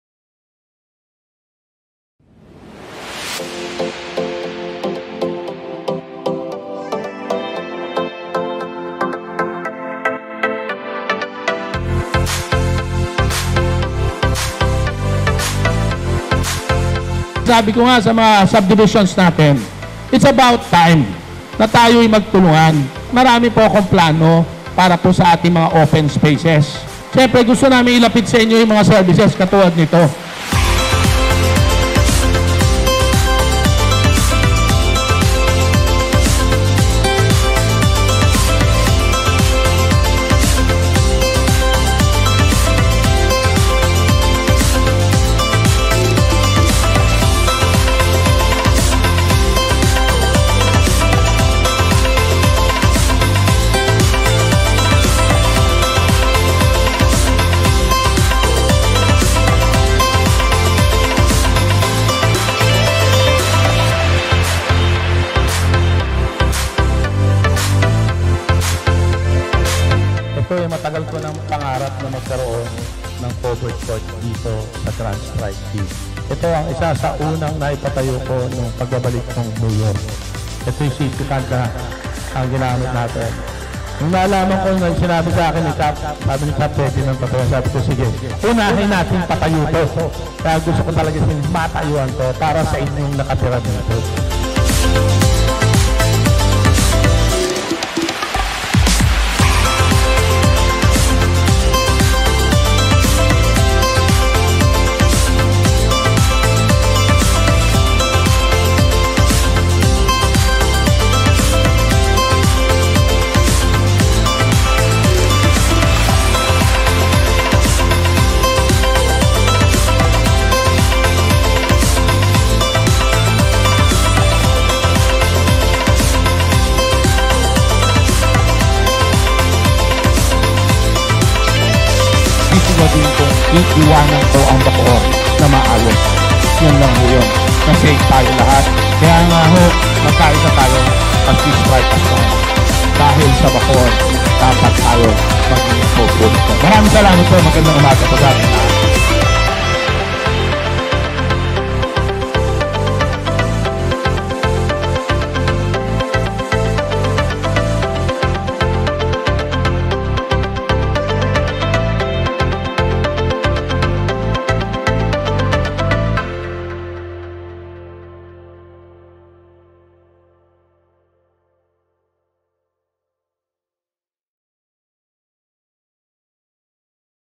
Blessing of Multi-Purpose Building sa Greentown Villas & Covered Court sa Grand Strikeville IV
May 20, 2023 - Pinangunahan ni Mayor Strike B. Revilla ang blessing ng dalawang nasabing establisyamento. Kung saan nabuo ang multi-purpose building, dahil sa inisyatiba ng ating mga butihing Mayor Strike Revilla, Vice Mayor Rowena Bautista – Mendiola, Cong. Lani Mercado – Revilla, Sangguniang Panlungsod Members at ni Brgy. Captain John Ernest C. Advincula.

Layunin ng proyektong ito na makapagbigay ng maayos at mabilis na serbisyo para sa ating mamayan. Hangad din ng ating Lungsod na matulungan ang ating bawat HOA Officers na sila’y maging mabisa at palagiang nakaalarma sa pagbibigay tulong sa bawat residente ng Greentown Villas.

Samantala,  pinangunahan din ng ating akalde ang Blessing sa Covered Court sa Grand Strikeville IV, Ikinatuwa naman ito ng lahat dahil napaparami natin ang mga lugar kung saan maaring ganapin ang pag-ensayo at paglalaro ng mga kabataan at atleta para sa kanilang mga hilig na sports lalo na sa larangan ng Basketball.